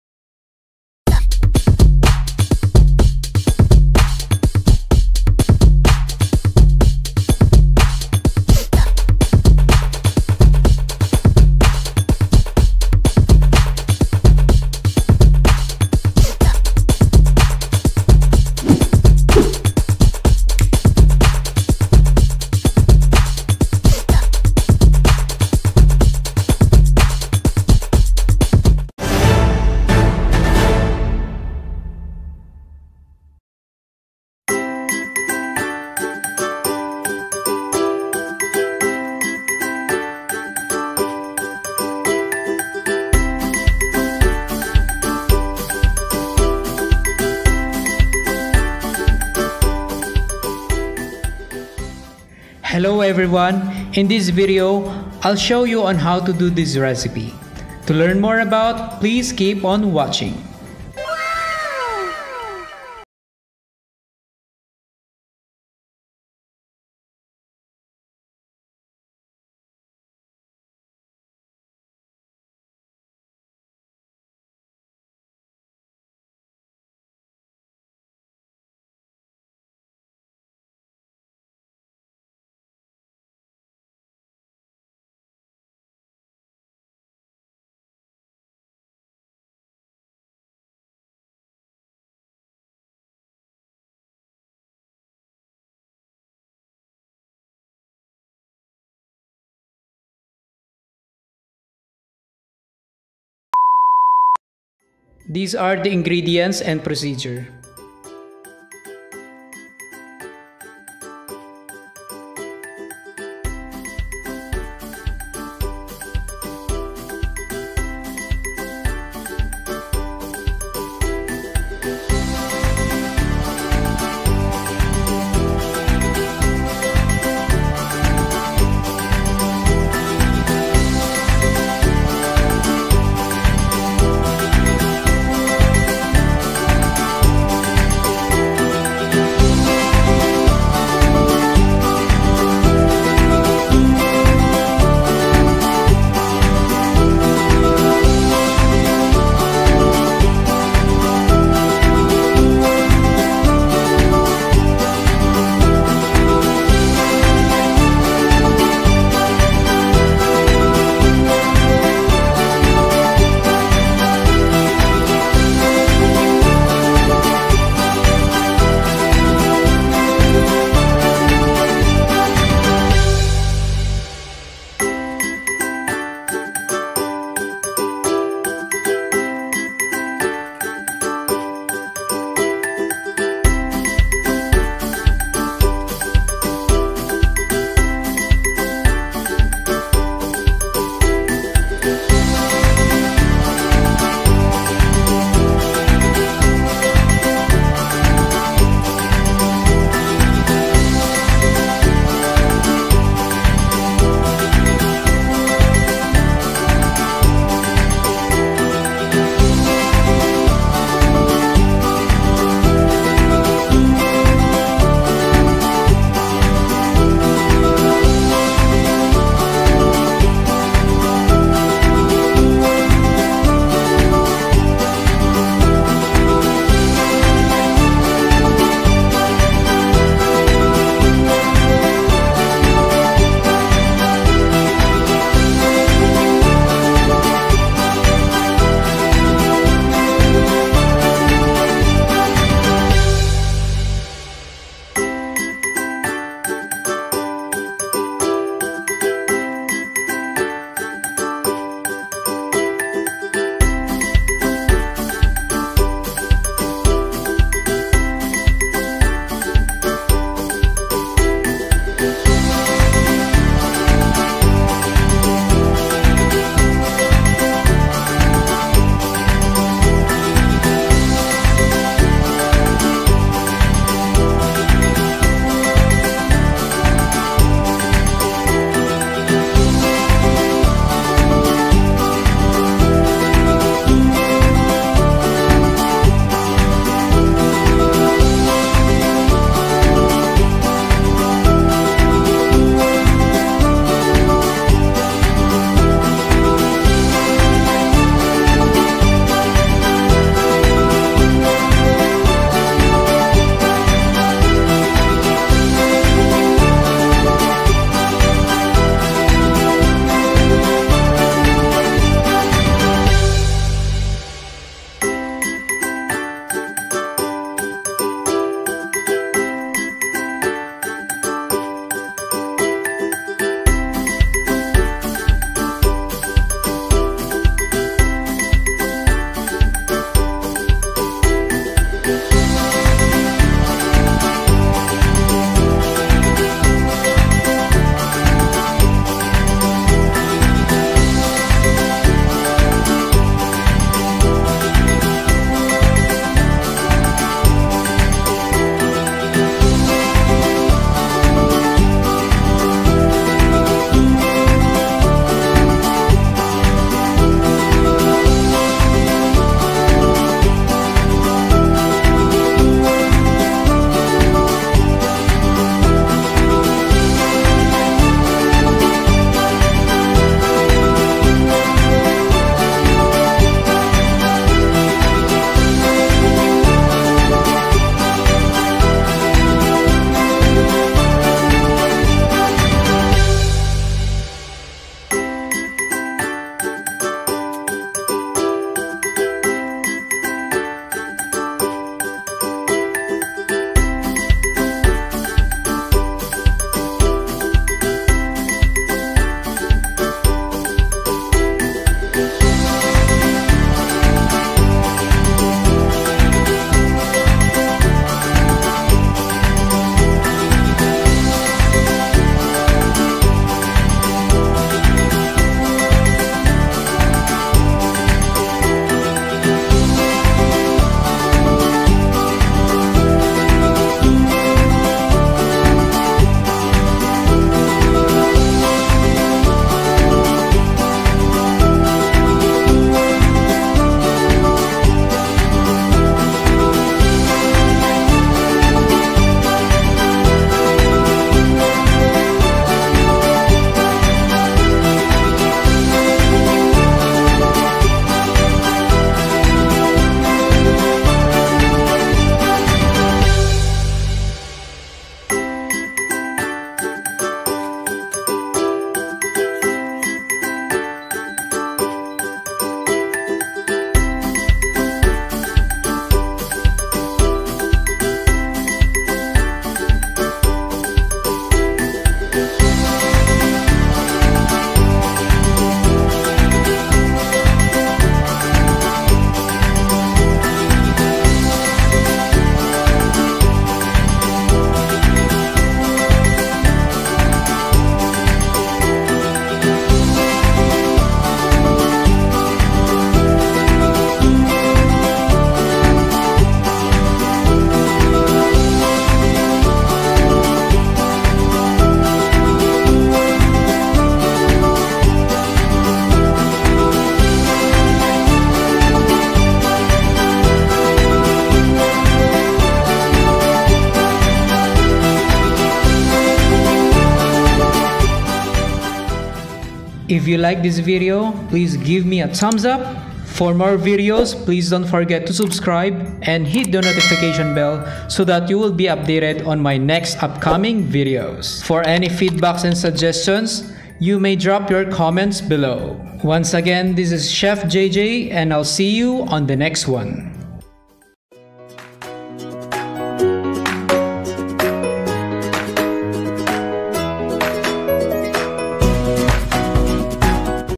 HOW TO BAKE PANDESAL | Chef JJ
Pandesal is a popular yeast-raised bread in the Philippines. Individual loaves are shaped by rolling the dough into long logs which are rolled in fine bread crumbs. These are then portioned, allowed to rise, and baked.

It is most commonly served hot and may be eaten as is, or dipped in coffee, tsokolate (hot chocolate), or milk. It can also be complemented with butter, margarine, cheese, jam, peanut butter, chocolate spread, or other fillings like eggs, sardines and meat.